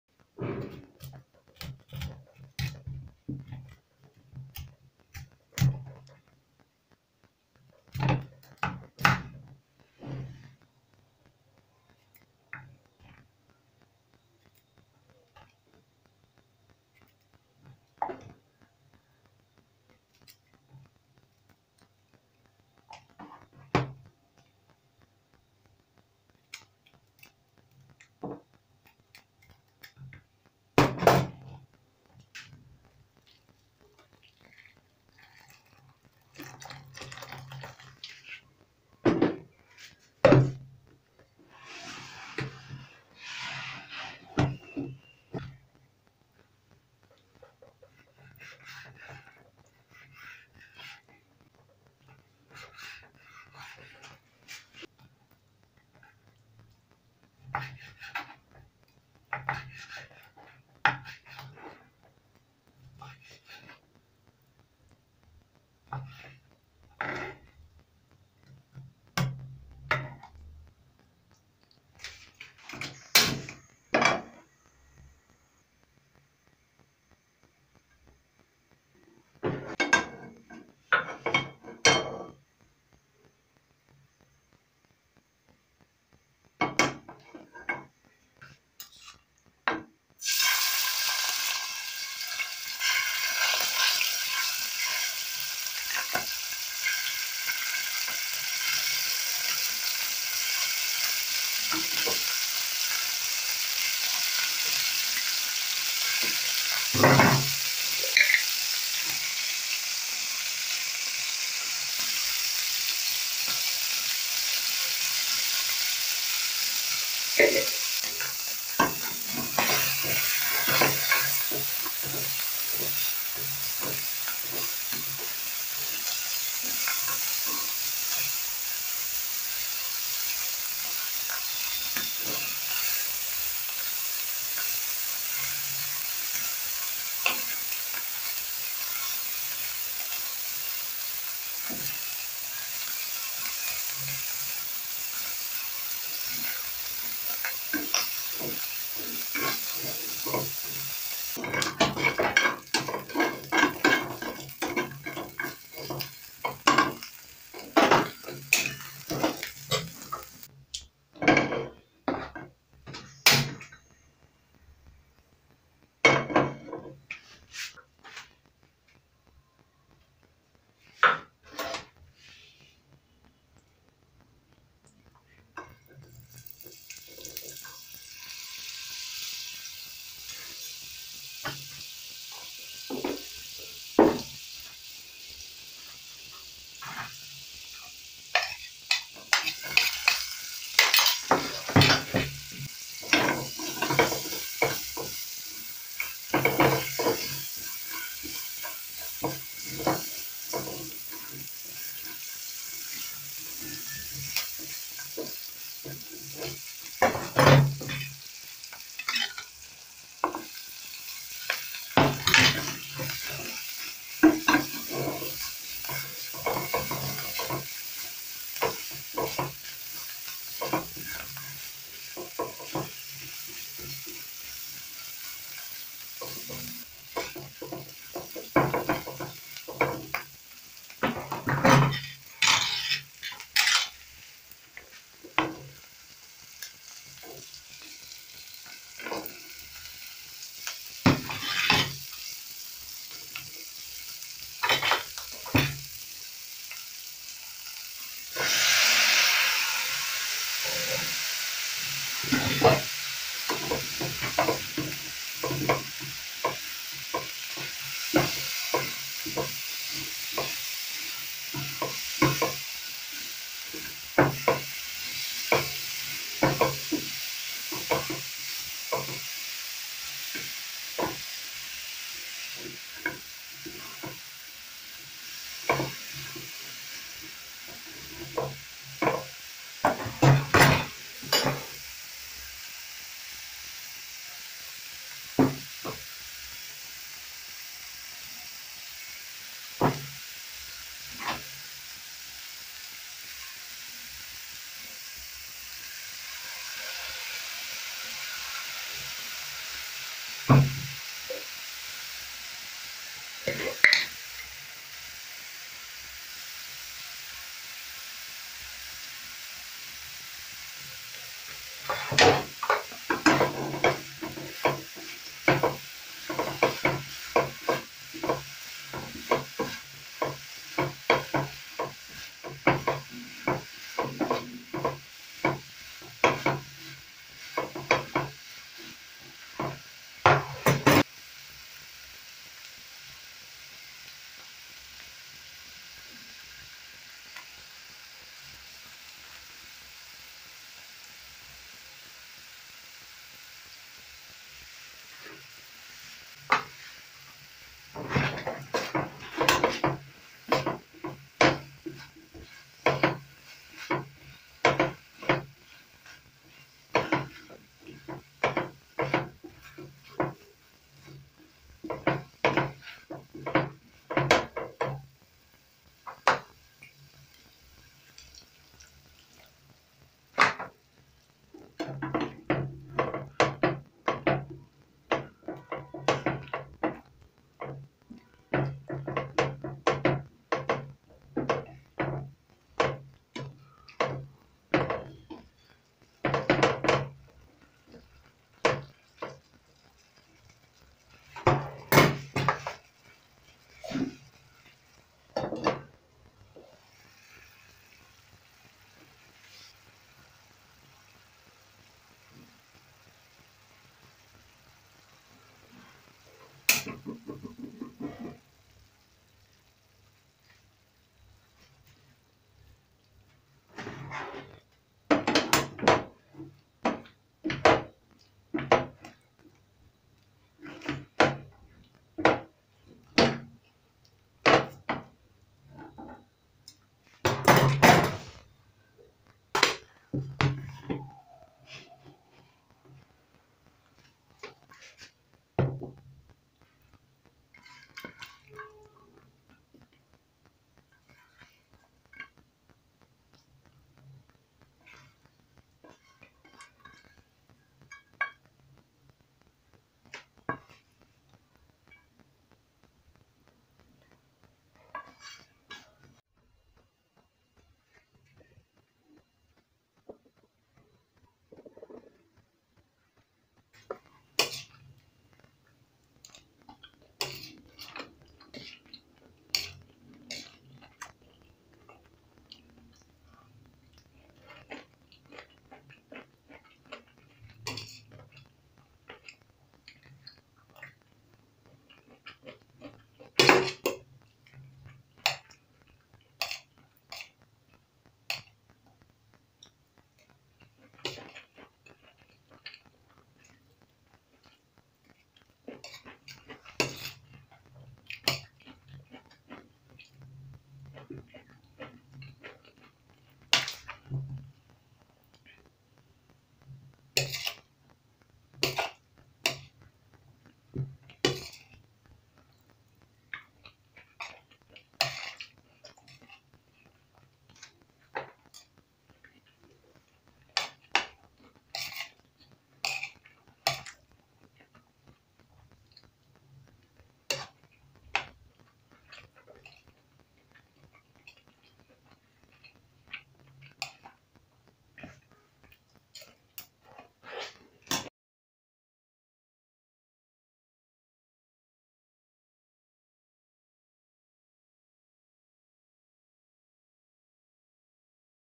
Chicken and Broccoli recipe || How to make Chicken and Broccoli stir fry
Broccoli and stir fry chicken recipe. Broccoli is an edible green plant in the cabbage family whose large flowering head, stalk and small associated leaves are eaten as a vegetable. Broccoli is classified in the Italica cultivar group of the species Brassica oleracea.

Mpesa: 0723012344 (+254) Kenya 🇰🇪